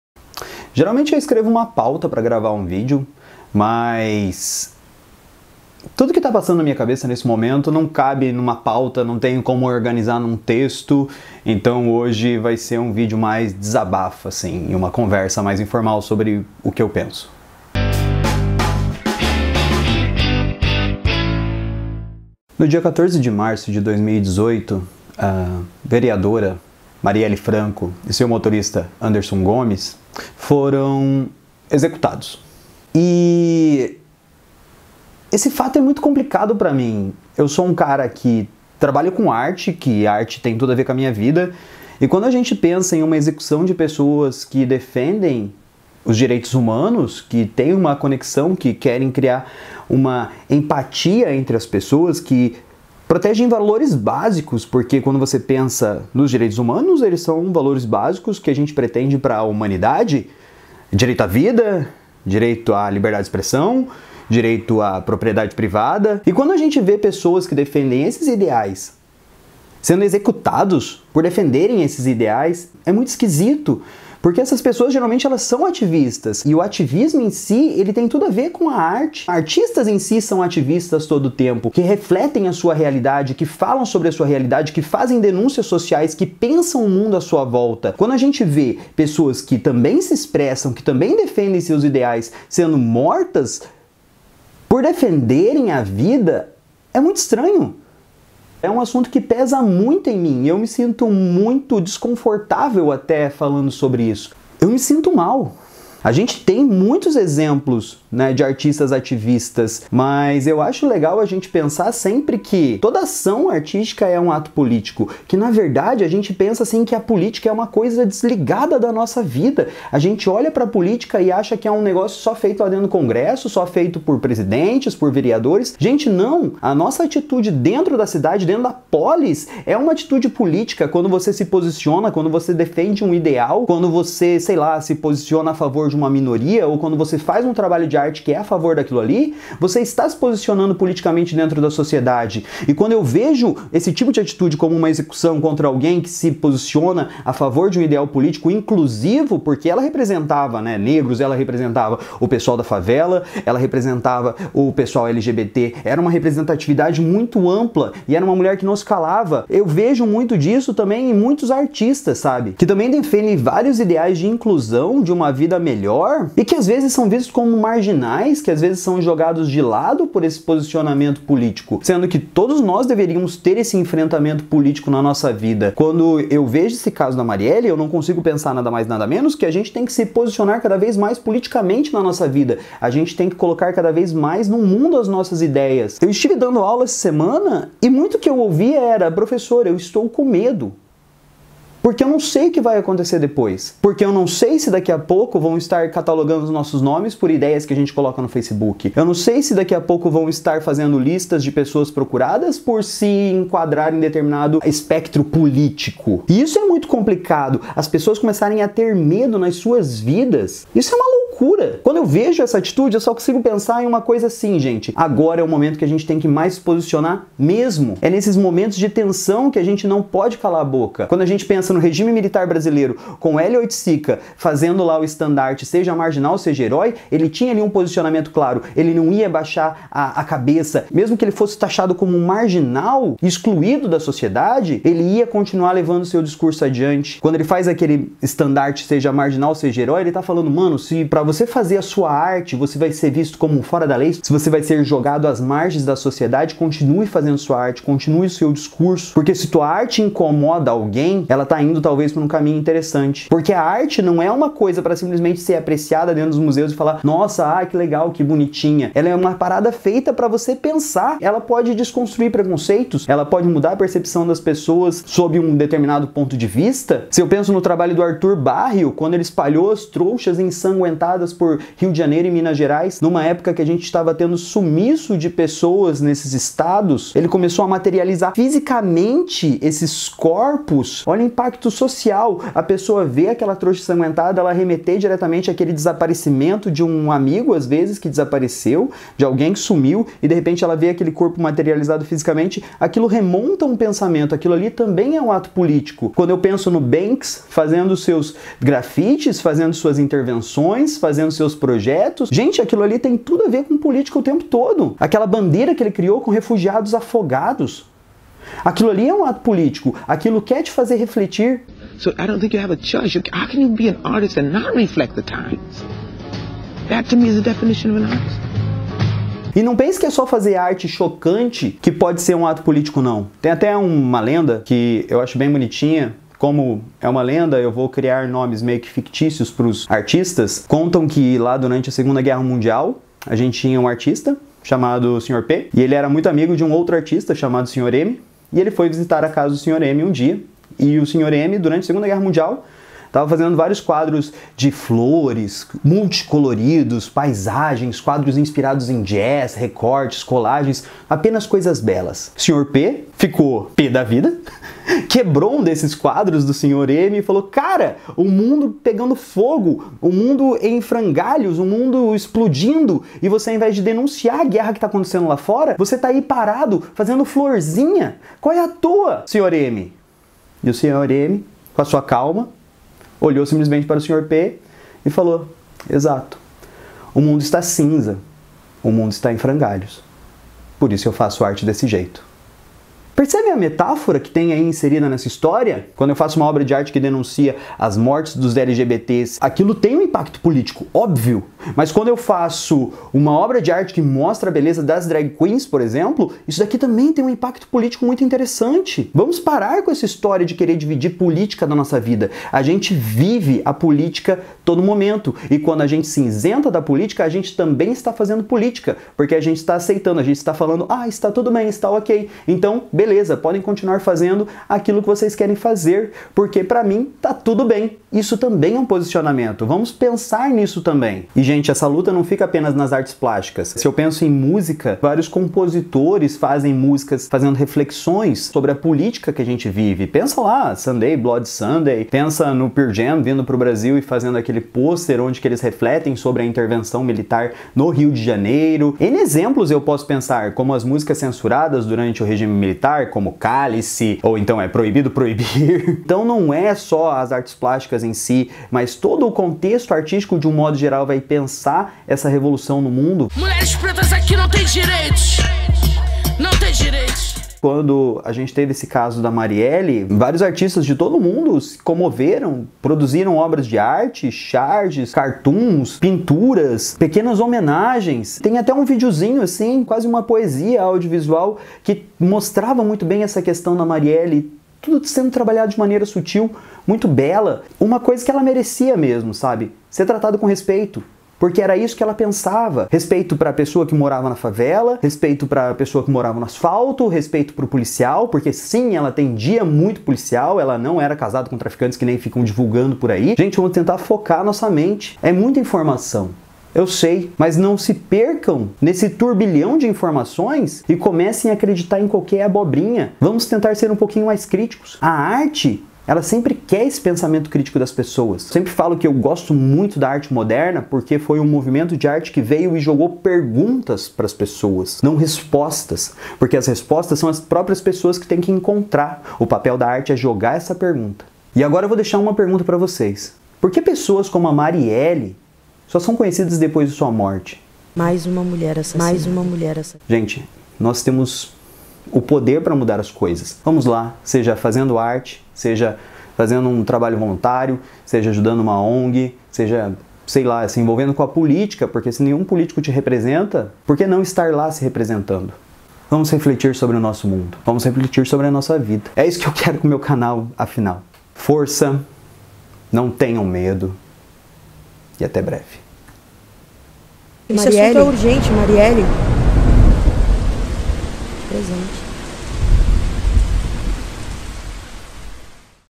Marielle Franco e a Arte como Ferramenta de Luta Política.
Esse vídeo é quase um desabafo sobre esse caso bárbaro pelo qual vivemos e ainda não conseguimos mensurar o impacto... Só sabemos que estamos diante de uma conjuntura onde apenas uma palavra se enquadra: LUTA!